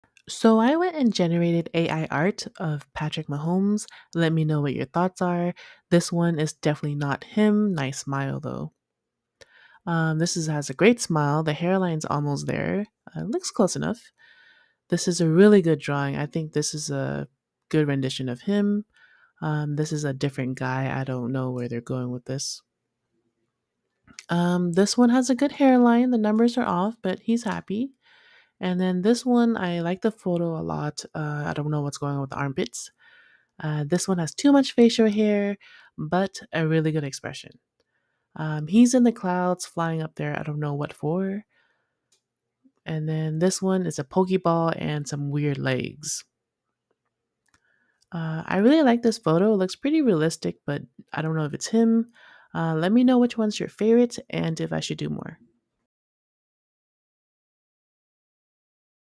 Patrick Mahomes AI Art?
In this video, we take a look at some AI-generated art I generated Kansas City's Quarterback Patrick Mahomes. From digital paintings to 3D renderings, we explore how good or how bad these photos are. Whether you're a fan of football or just interested in the intersection of art and technology, this video is for you.